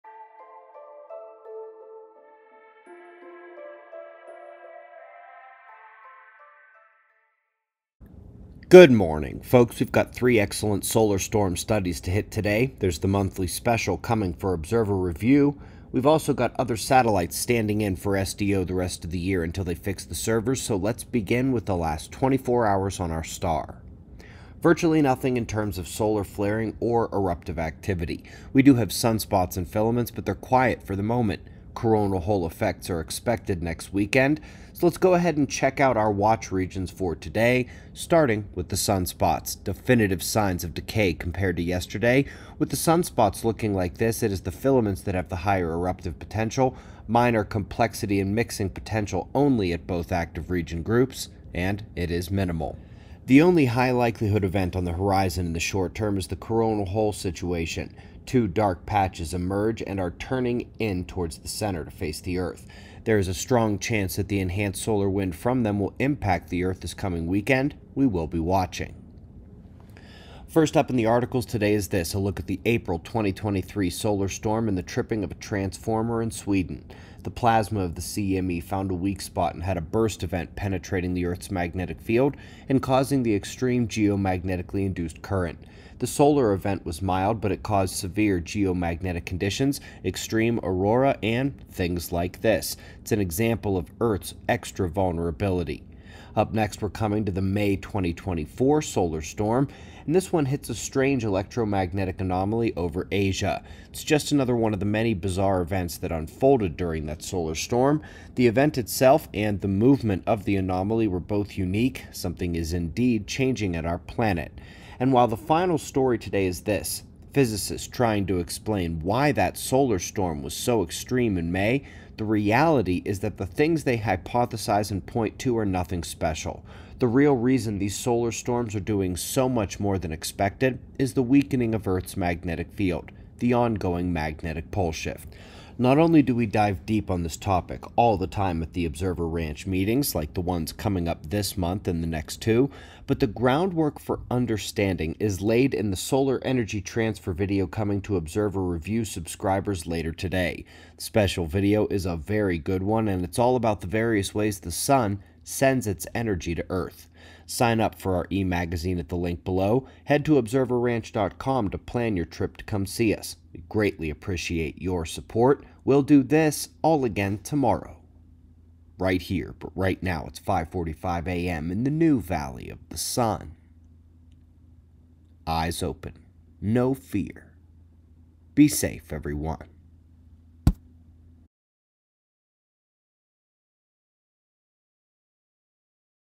Solar Watch, Solar Storm Anomalies & the Pole Shift | S0 News Dec.2.2024
(Special Video Coming Today)